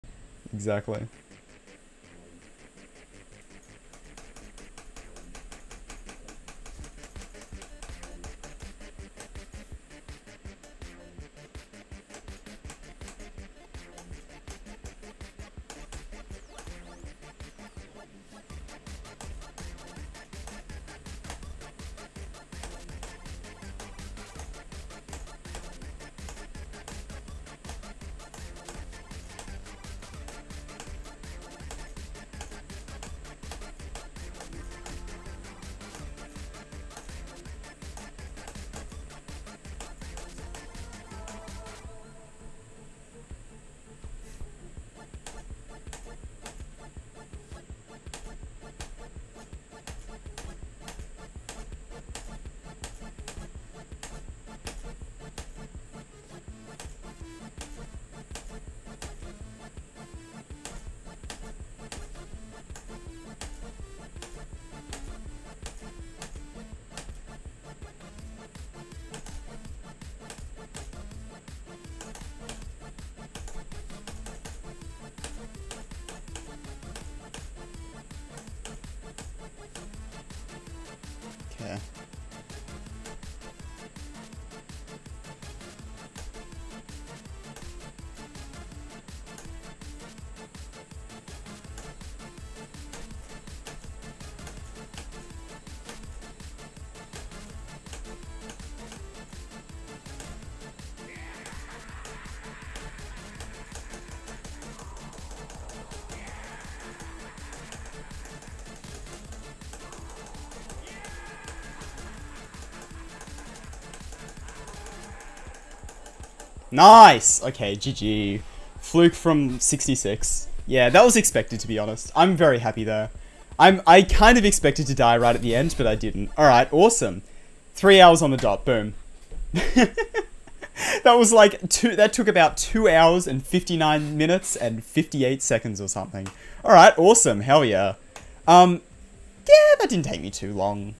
Puzzle trials 100% (Insane demon)
Really surprised I one streamed this one, it's a level that was suggested to me ages ago that I've only just now gotten around to beating. Overall it's a decent level, definitely not bad by any stretch but not the best thing I've ever played. Decently fun, very unique which I like